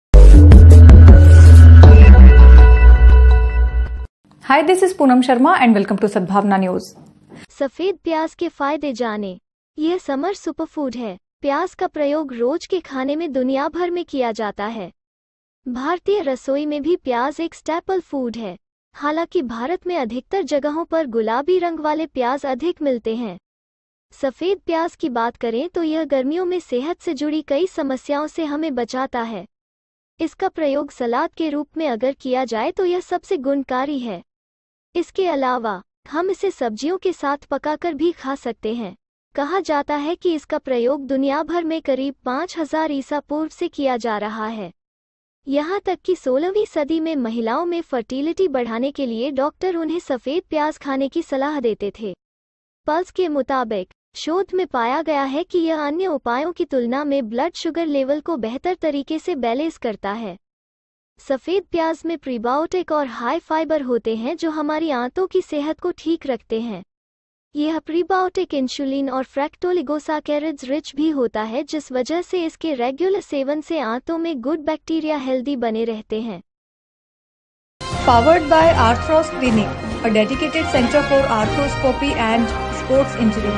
सफेद प्याज के फायदे जानें यह समर सुपरफूड है,Know the benefits of white onion, this summer superfood
Health Benefits Of White Onion: प्‍याज (White Onion) का प्रयोग रोज के खाने में दुनियाभर में किया जाता है. भारतीय रसोई में भी प्‍याज एक स्‍टेपल फूड है. हालांकि भारत में अधिकतर जगहों पर गुलाबी रंग वाले प्‍याज अधिक मिलते हैं. सफेद प्‍याज की बात करें तो यह गर्मियों में सेहत (Summer Health) से जुड़ी कई समस्‍याओं से हमें बचाता है. इसका प्रयोग सलाद के रूप में अगर किया जाए तो यह सबसे गुणकारी है. इसके अलावा, हम इसे सब्जियों के साथ पकाकर भी खा सकते हैं. कहा जाता है कि इसका प्रयोग दुनियाभर में करीब 5000 ईसापूर्व से किया जा रहा है. यहां तक कि 16वीं सदी में महिलाओं में फर्टीलिटी बढ़ाने के लिए डॉक्‍टर उन्‍हें सफेद प्‍याज खाने की सलाह देते थे. पल्‍स के मुताबिक, शोध में पाया गया है कि यह अन्‍य उपायों की तुलना में ब्‍लड शुगर लेवल को बेहतर तरीके से बैलेंस करता है.

डाइजेशन के लिए बेहतर

सफेद प्‍याज में प्रीबायोटिक और हाई फाइबर होते हैं जो हमारी आंतों की सेहत को ठीक रखते हैं. यह प्रीबायोटिक इंश्‍यूलीन और फ्रैक्‍टोलिगोसाकैरिड्स रिच भी होता है जिस वजह से इसके रेग्‍युलर सेवन से आंतों में गुड बैक्‍टीरिया हेल्‍दी बने रहते हैं.
Health Benefits Of White Onion: Onion (White Onion) is used worldwide in daily food. Onion is a staple food in Indian kitchen. Although pink colored onions are found in most places in India. Talking about white onion, it protects us from many summer health problems. It is most effective if used as a salad. Apart from this, we can also cook it with vegetables and eat it. It is said that it is being used around 5000 BC all over the world. Even in the 16th century, doctors used to advise them to eat white onion to increase the fertility in women. According to Puls, research has found that it balances blood sugar levels better than other measures.

Better for design

White onion contains prebiotic and high fiber which keeps our intestinal health right. It is also rich in prebiotic insulin and fractoligosaccharides, due to which its regular intake helps maintain good bacteria in the intestines.